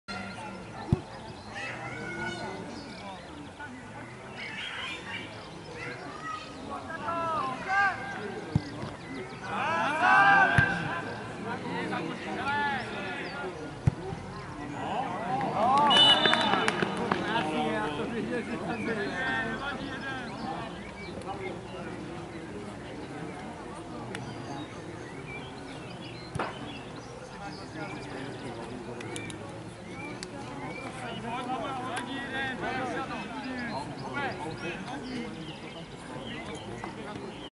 Stod - Horšovský Týn 7:3 - Gól Horšovského Týna na 3:1
Jarní turnaj - 4. zápas - sezona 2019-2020
Stod - Horšovský Týn
Gól Horšovského Týna na 3:1